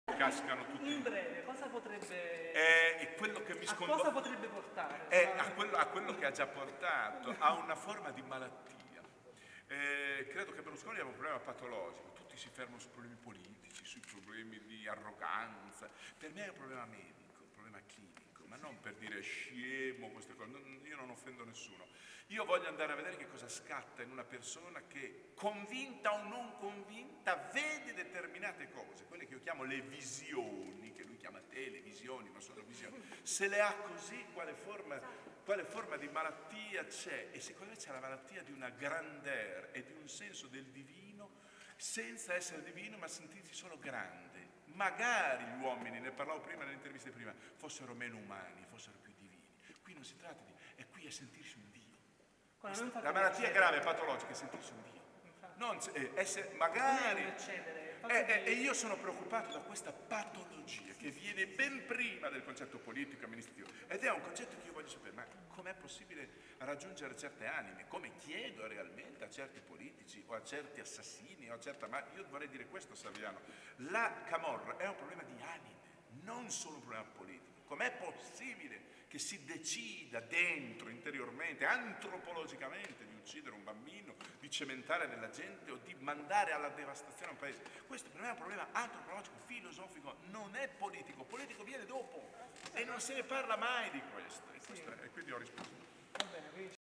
Alessandro Bergonzoni-Press Conference Salerno 2009
Ultimo passaggio della conferenza stampa del maestro Bergonzoni,al Festival Linea d'Ombra di Salerno,festival che contava su 60 eventi in 6 giorni.Il Caos nel cinema, nella musica, nella performing art e nella scrittura.